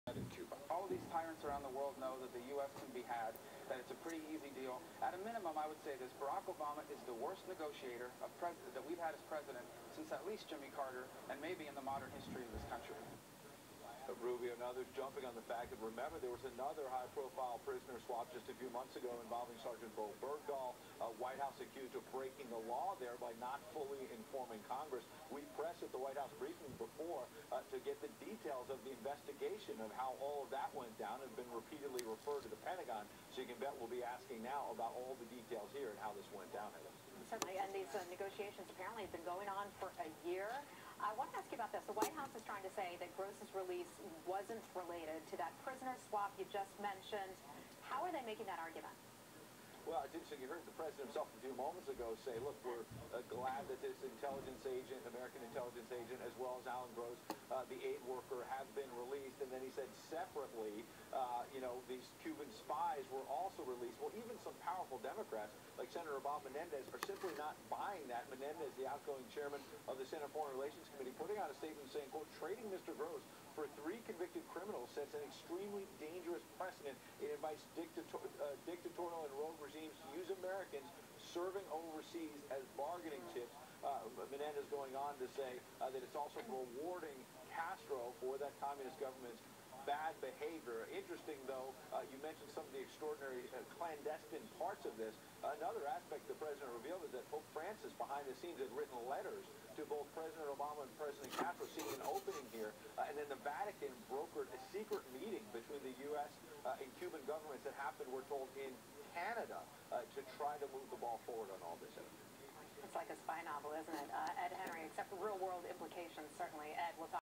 Reaction to CUBA releasing ALAN GROSS
Reaction, immediately following the announcement of Cuba's decision to release Alan Gross on the Fox News Channel.